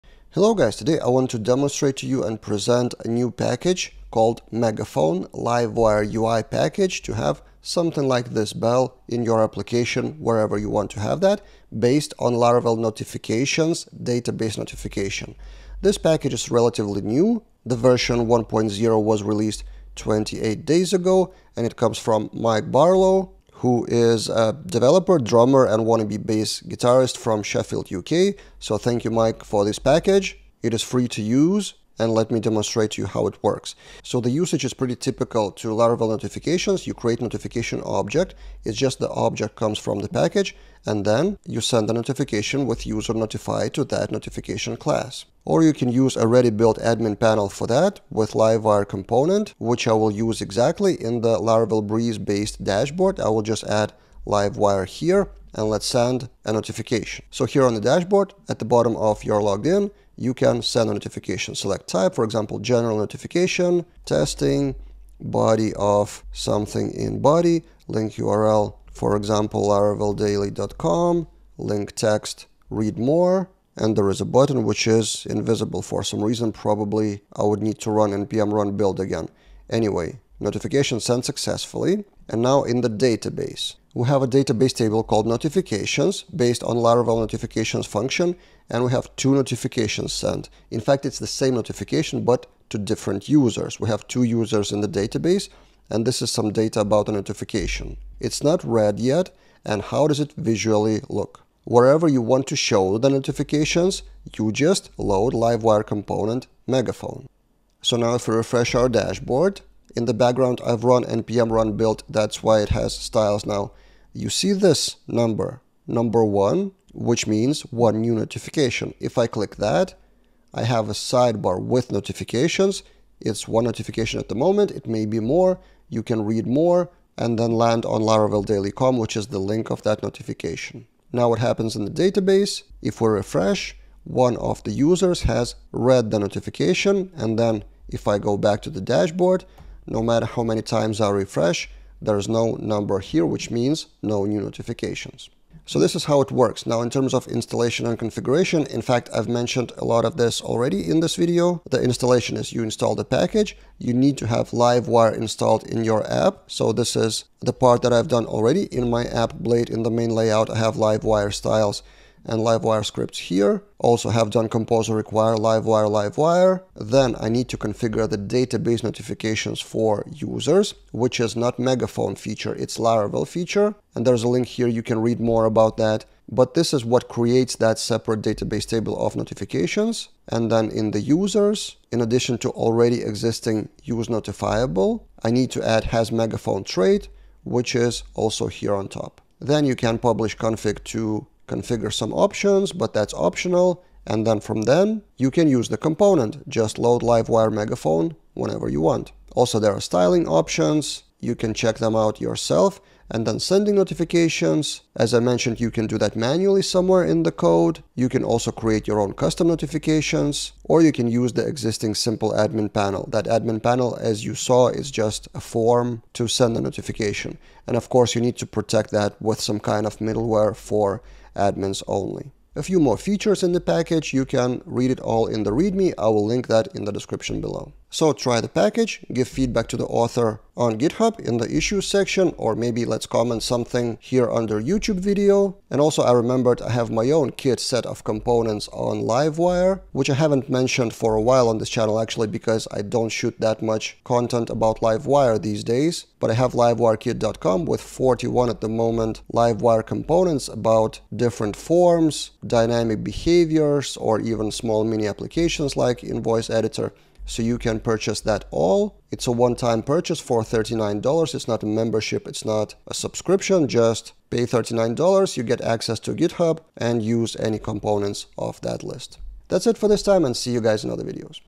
New Laravel UI Megaphone Package for "Bell" Notifications
Today I want to demonstrate to you a package to send and show the database notifications to your Laravel users.